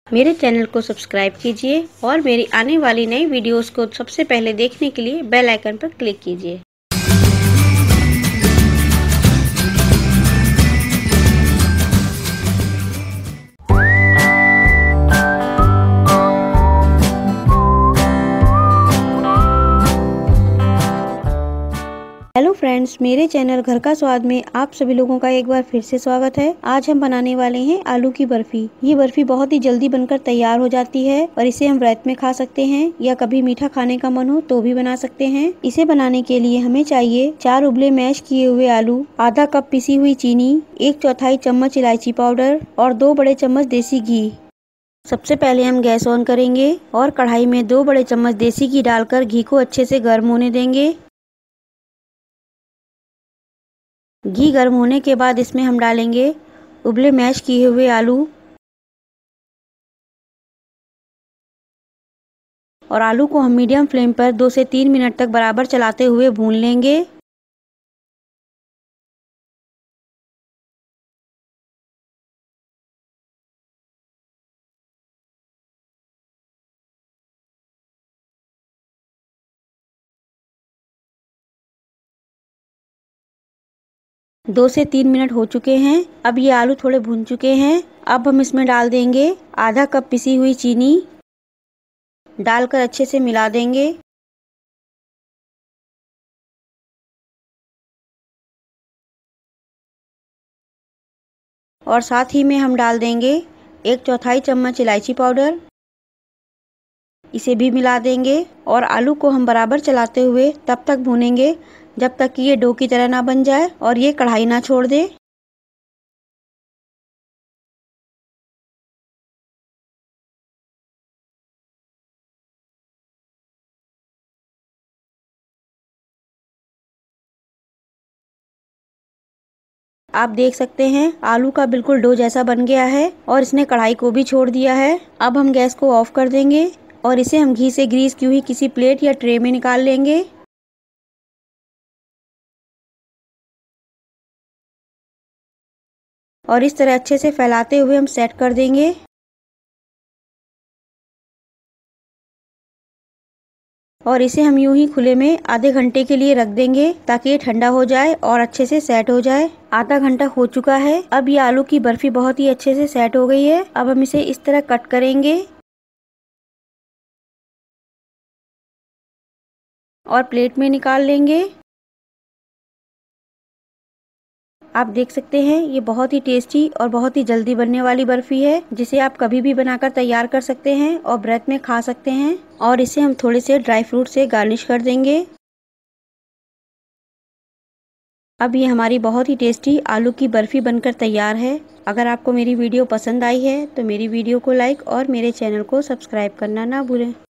नवरात्री व्रत में बनाये 2 चीज़ो से यह स्वादिष्ट बर्फी मिनटो मे | Falahari Barfi Recipe |Ghar ka Swad
आज मैं आपके साथ आलू बर्फी की रेसिपी शेयर करने जा रही हूँ |